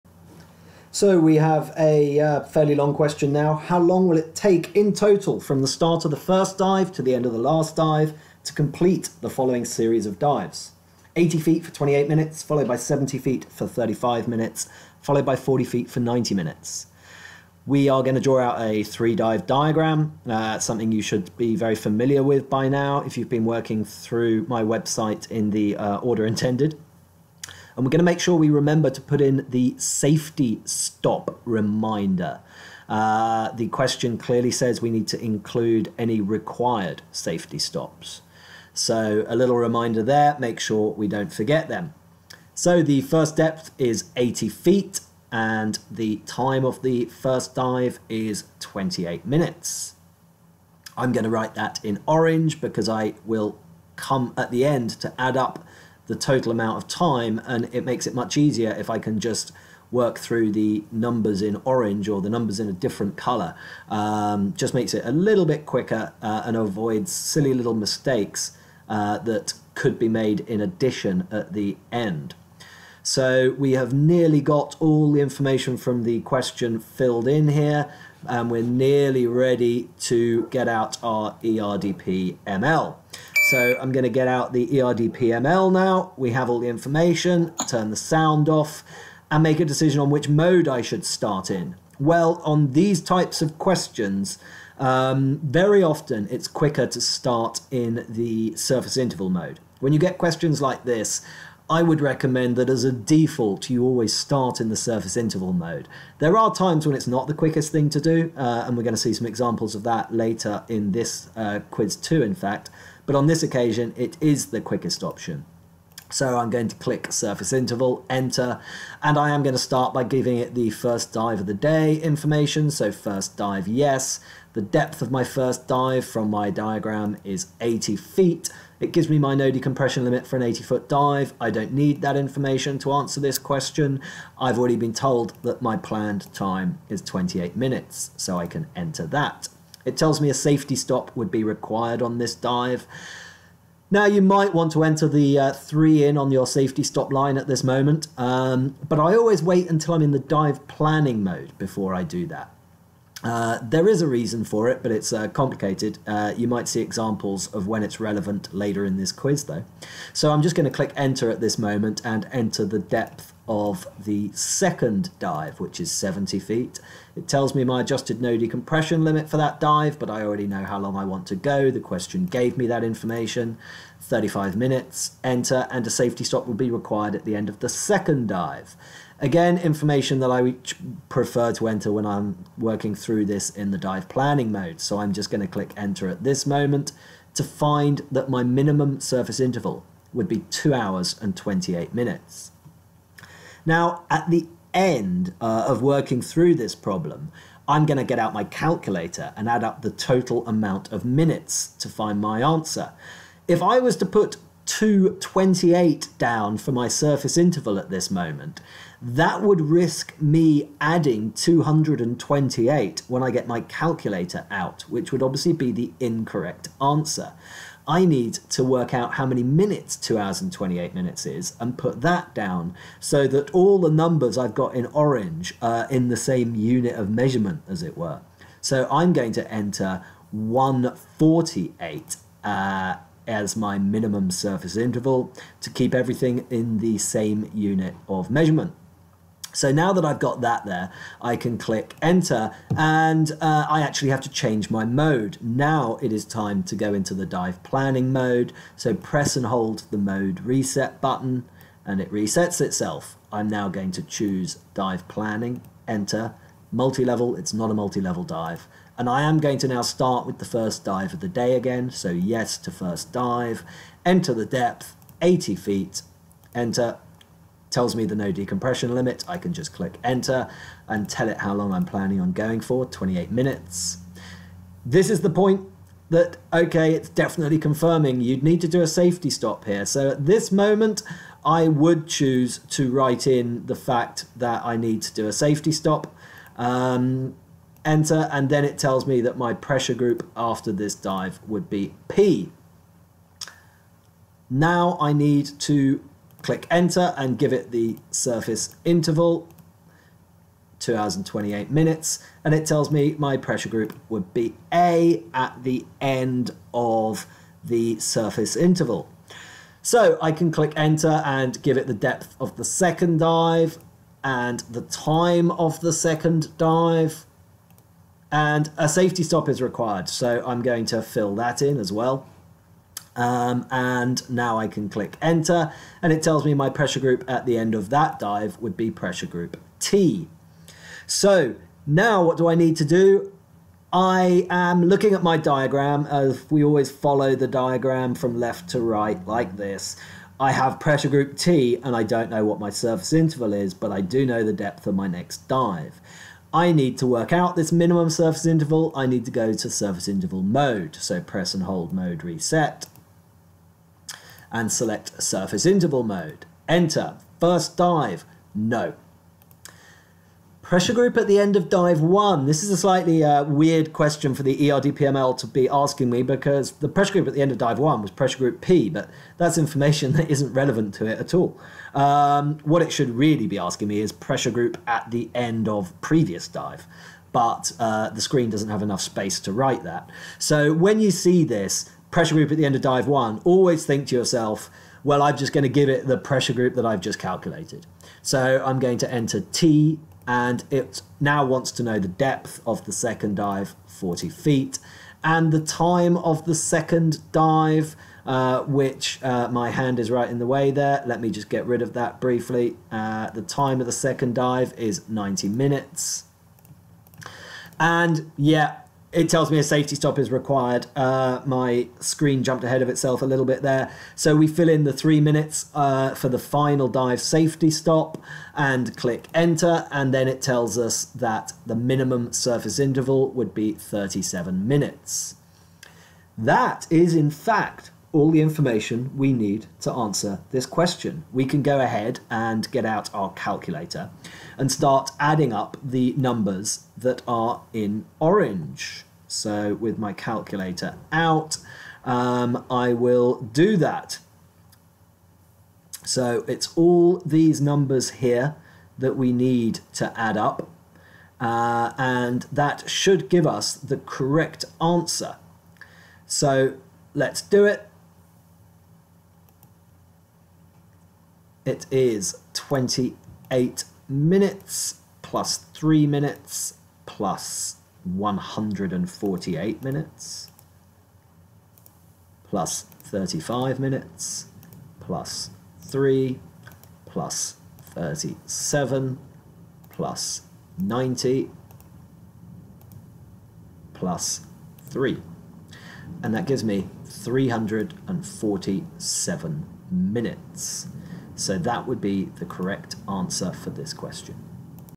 PADI Divemaster and Instructor exams, ERDPml Quiz 2 Question 3
Recreational dive planner, PADI Instructor Development Course, IE and Divemaster Theory final exam 2.  Quiz 2 q3